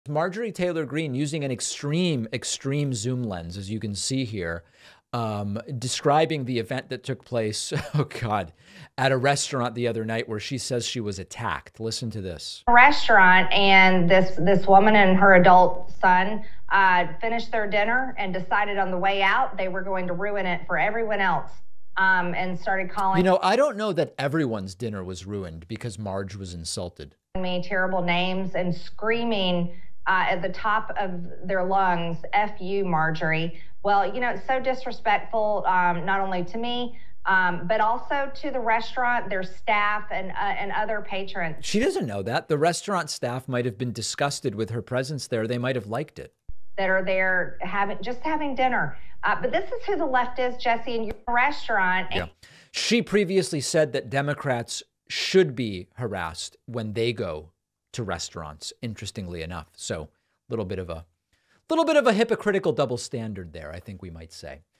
Marjorie Taylor Greene Humiliated at a Restaurant
MTG recounts her story of being confronted at a restaurant.